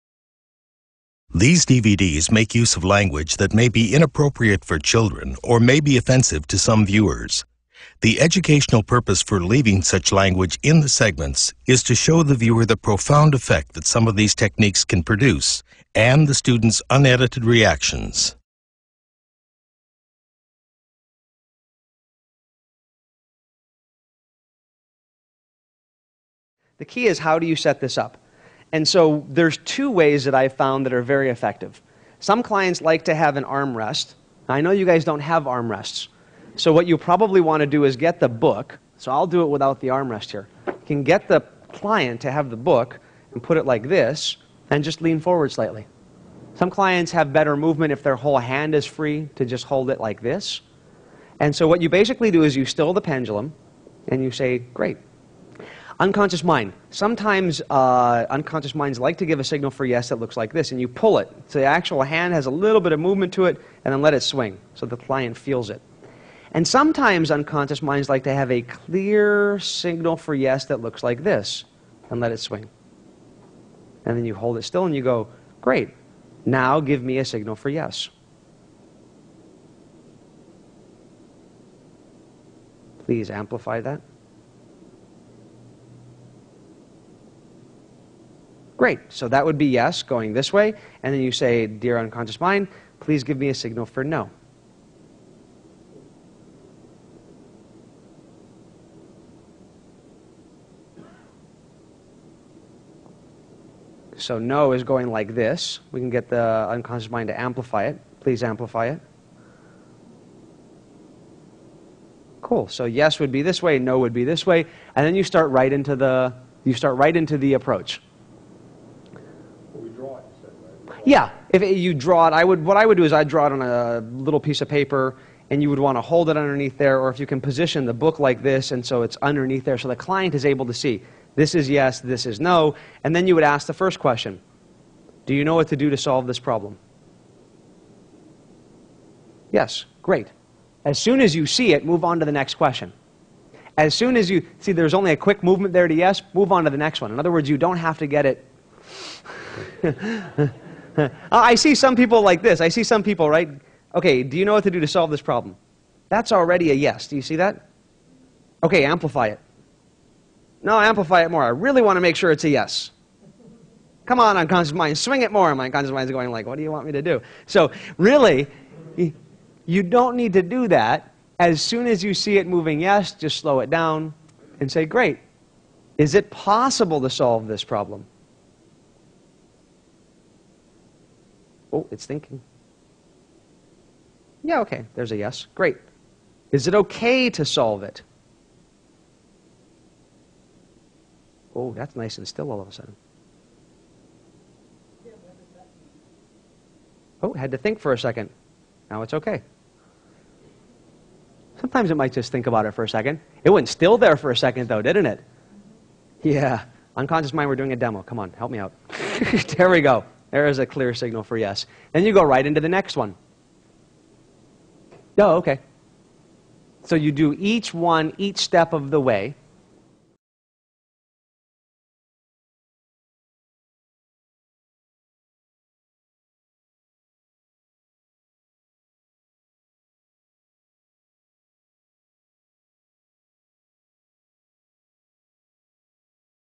Dr.Matt's NLP Techniques: Pendulum
Watch as Dr. Matthew B. James teaches one of his students at a recent Neuro Linguistic Programming Training about the technique of the Pendulum.